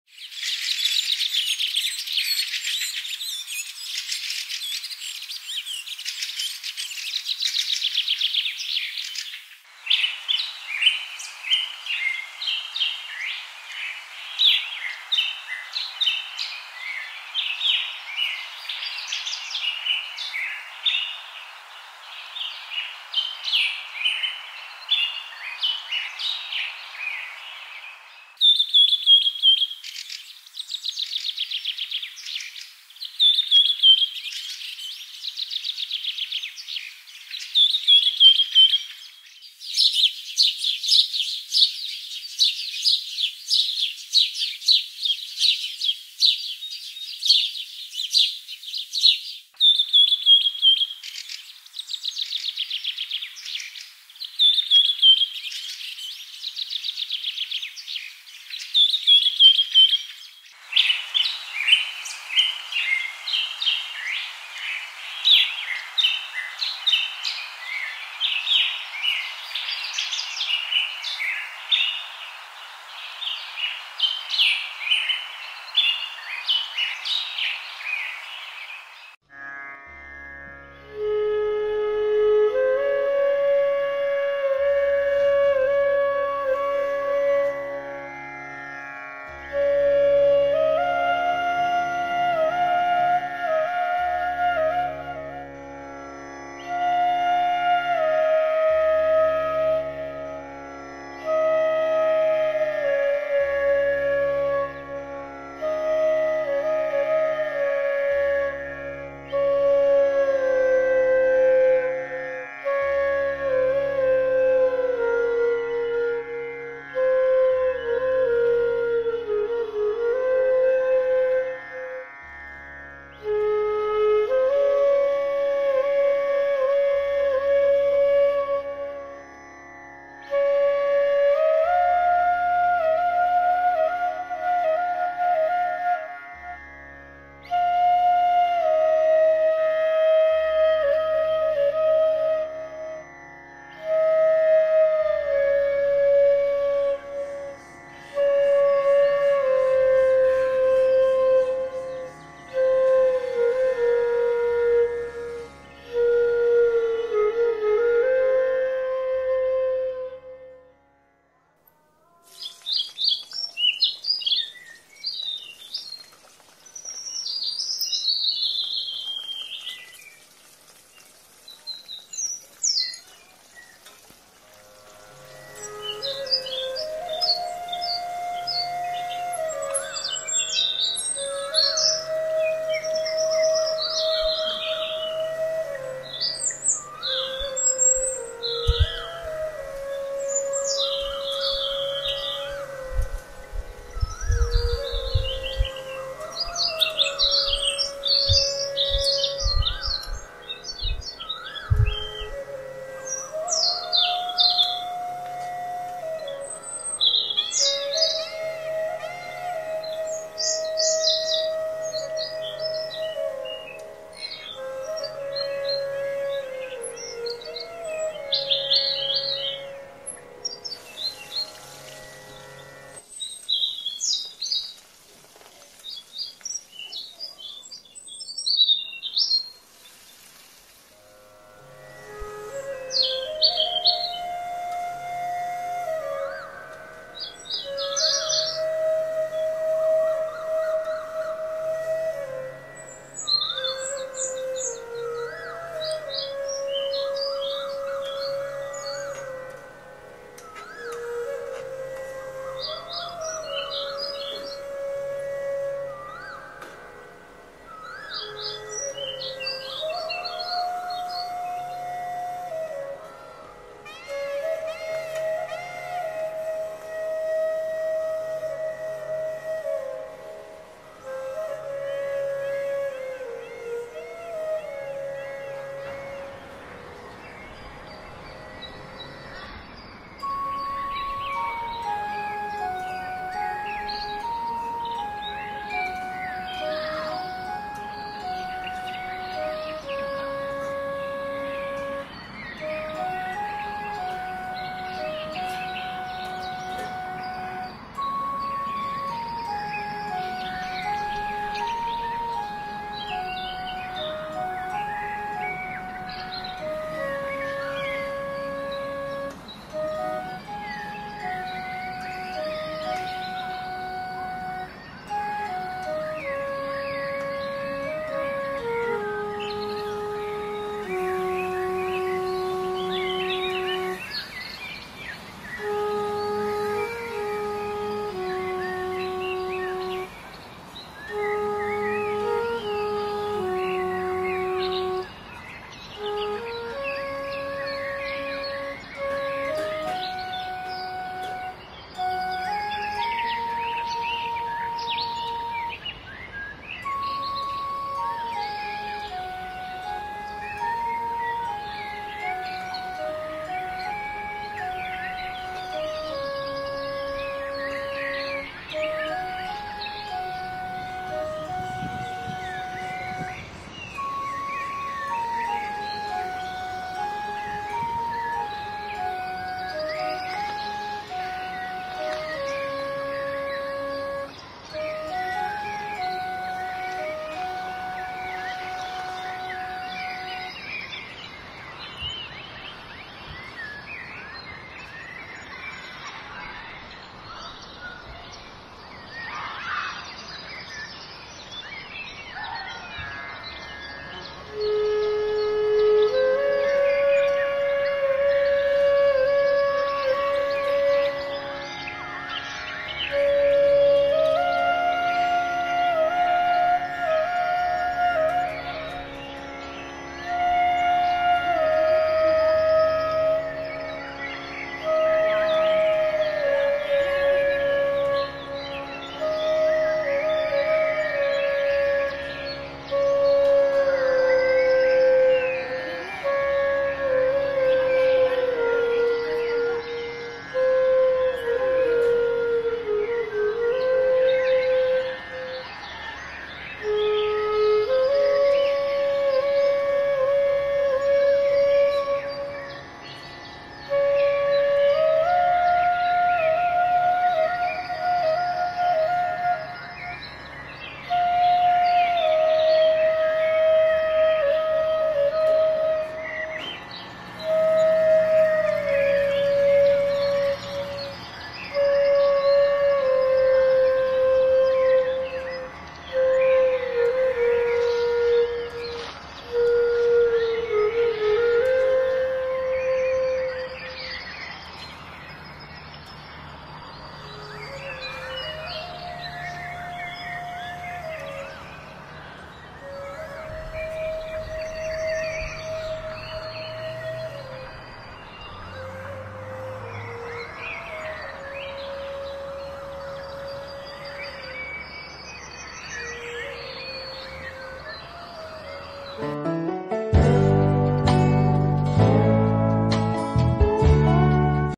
বাৰিতে মাৰিম পাক, বিচাৰি আনিম শাক,চজিনাৰ চৰ্চৰি Drumstick Curry Assamese Recipe
বাৰিতে মাৰিম পাক বিচাৰি আনিম শাক...
চজিনাৰ চৰ্চৰি
Drumstick Curry
Assamese Recipe
Healthy Drumstick Curry
Village Style Drumstick Curry